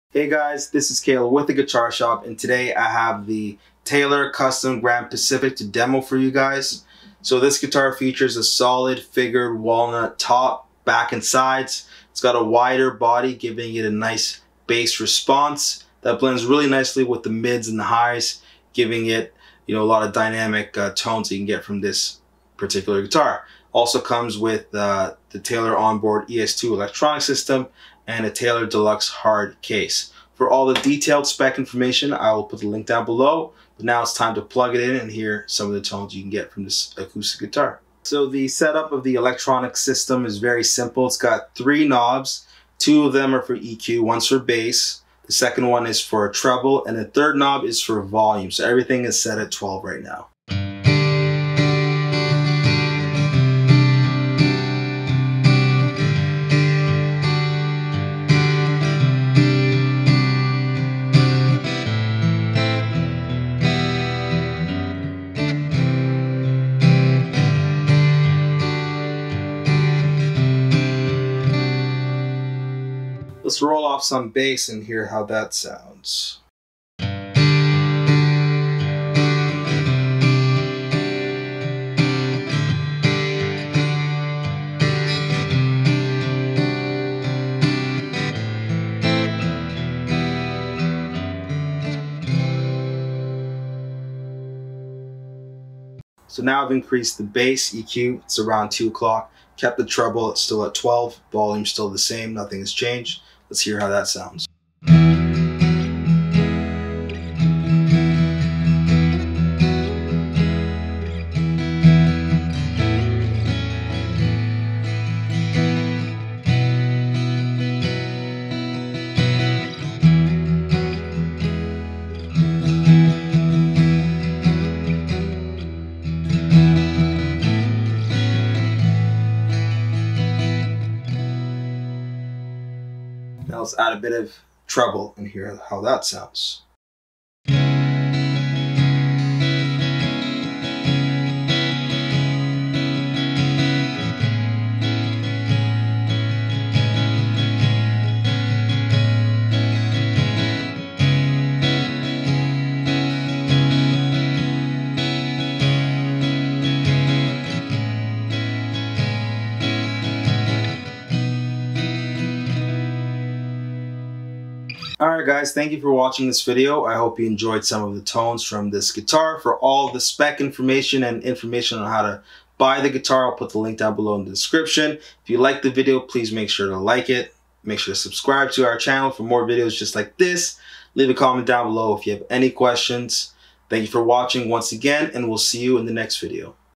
TAYLOR CUSTOM GRAND PACIFIC WALNUT CATCH #38 Demo The Guitar Shop
Here is Kale demoing this beautiful sounding Taylor Custom Grand Pacific Walnut acoustic guitar.